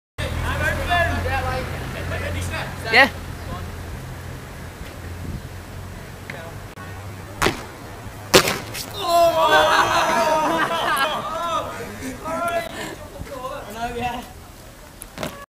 Corey's Funny Bail
just me josh and corey doing a like combo thing 4 camera and then corey has 2 go and bail but it was funny